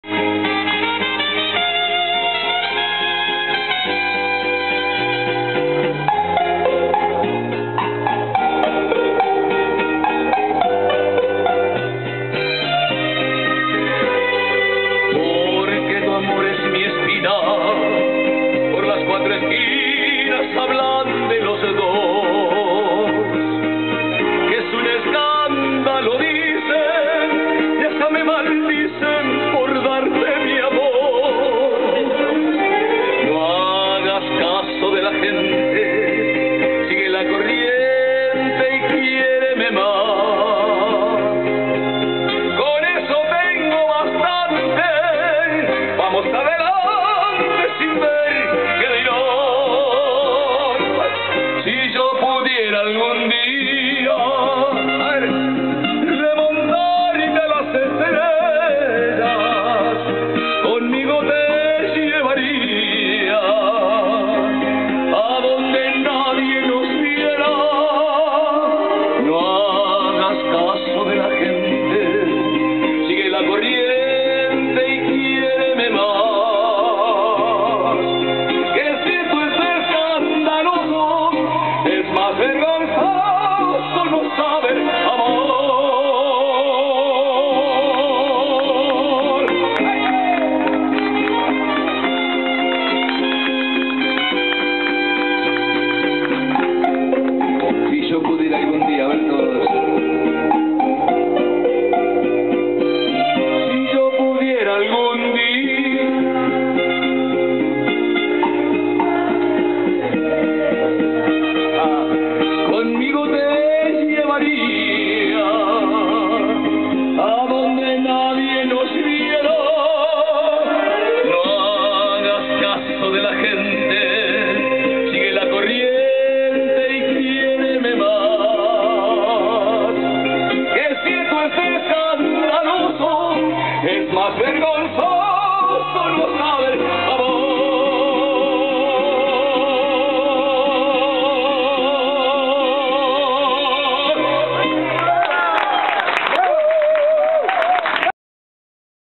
Jorge Zàrate-2
Cantando en el restaurante Carletto de Puerto Madero el domingo 20 de noviembre de 2016